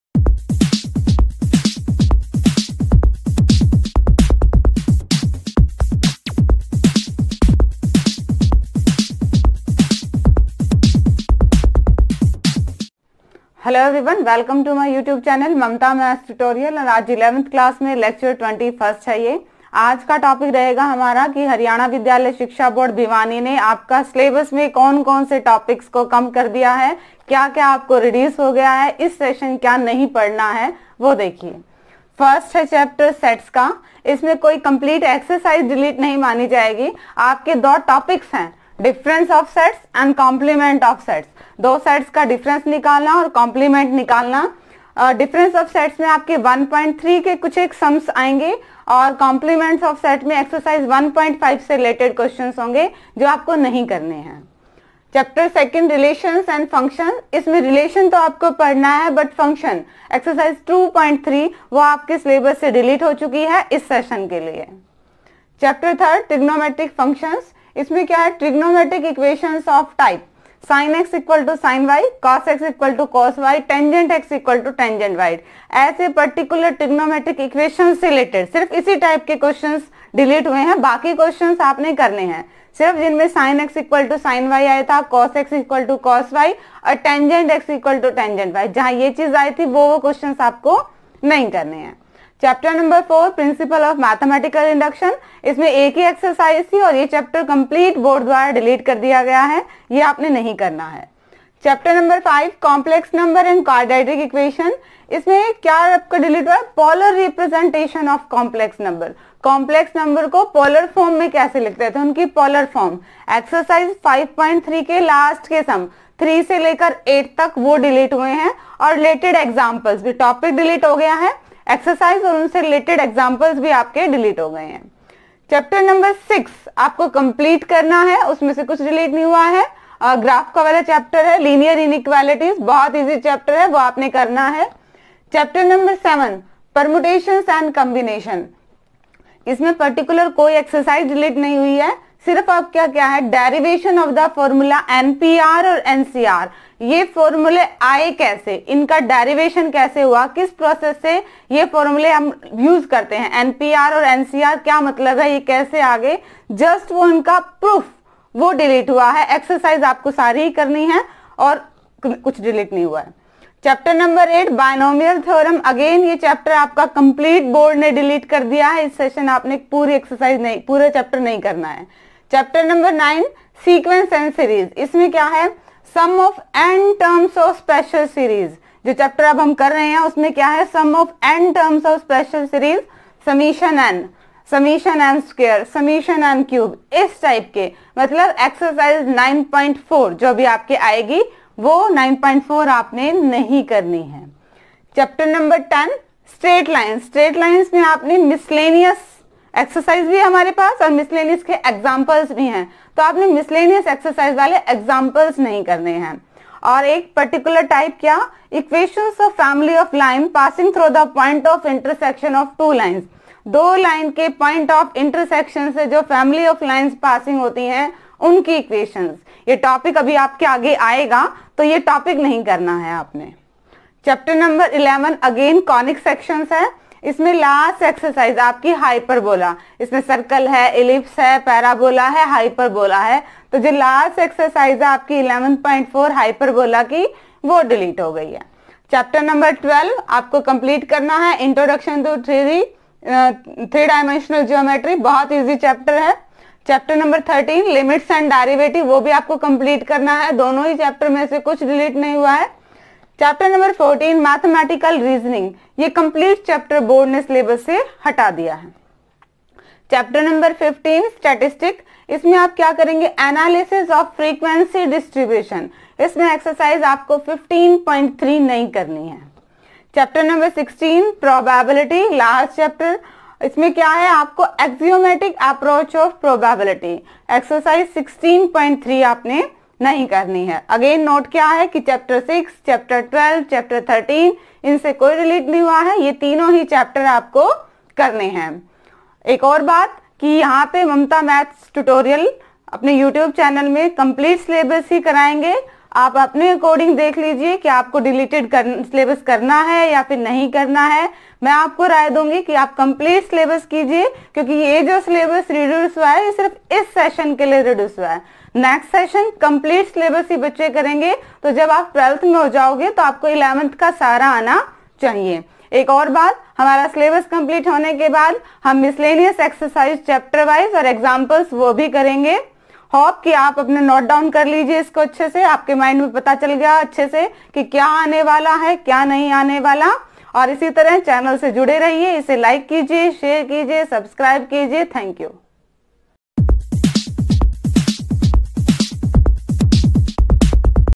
Deleted Syllabus/ Reduced syllabus of Maths of Class 11 (HBSE )
Here is link for information regarding mathematics reduced syllabus/ Deleted syllabus by Haryana Board for class 11👇👇

For 9th class mathematics Deleted syllabus